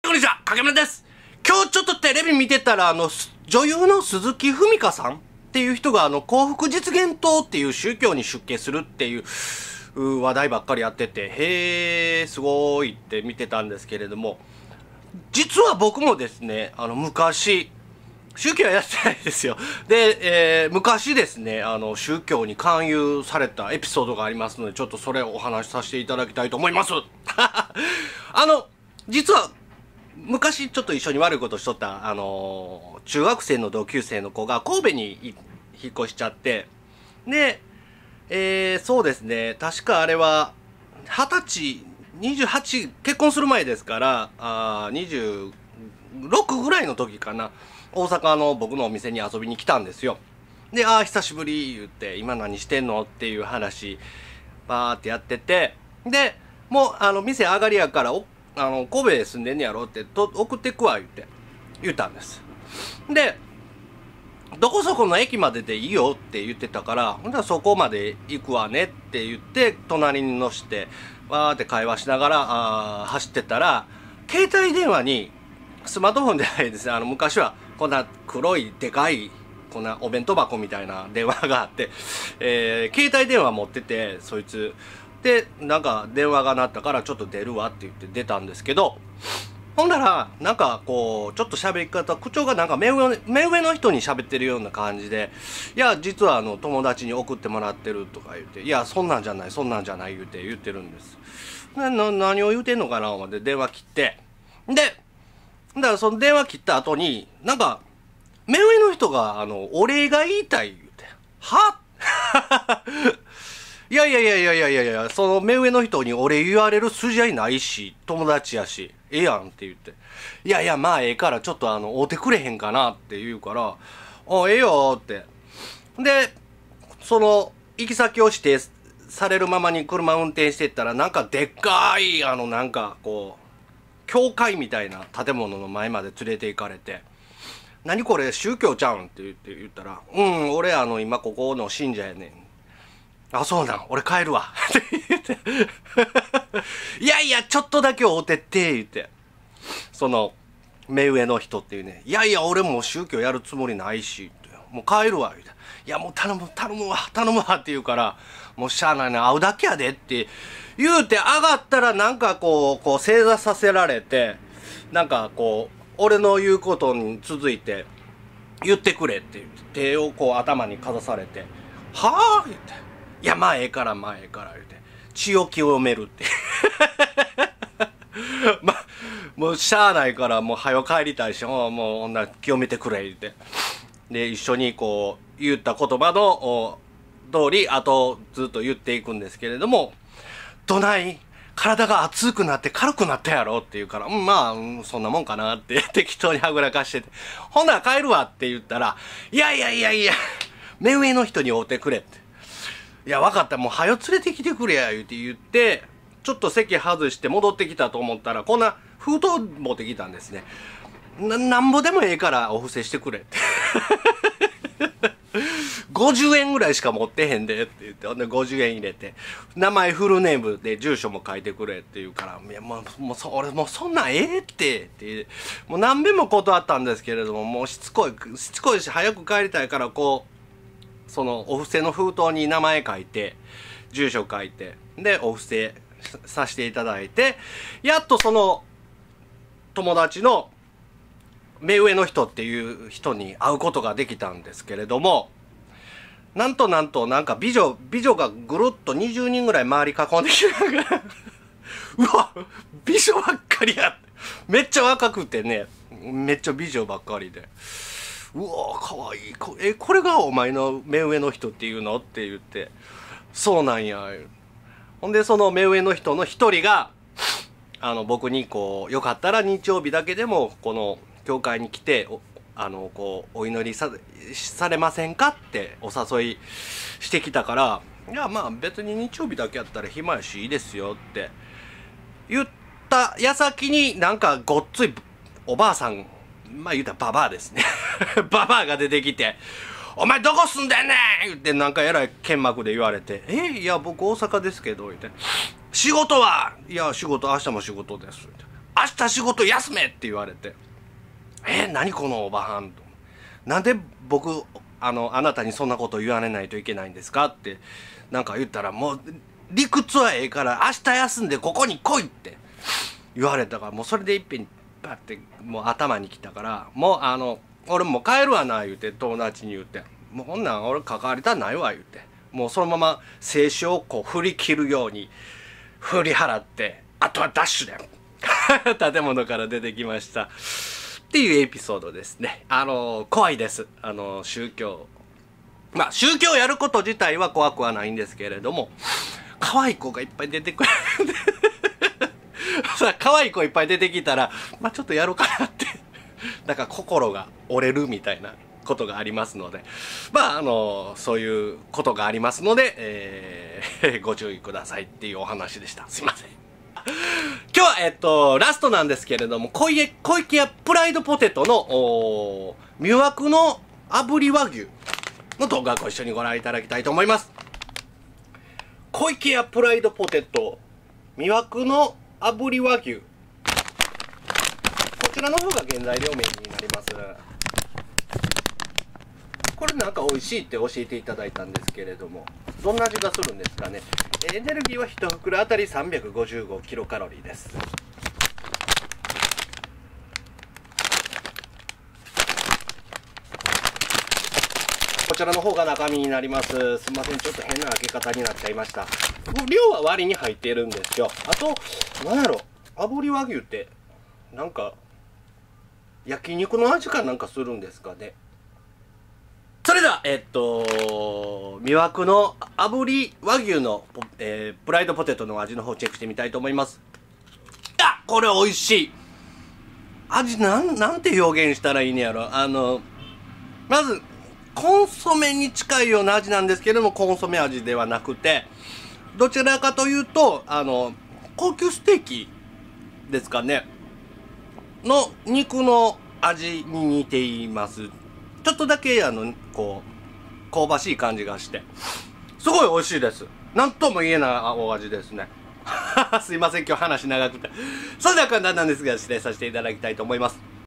コイケヤ プライドポテト 魅惑の炙り和牛 レビュー
コイケヤ プライドポテト 魅惑の炙り和牛味を購入し食べた感想を解説した動画です。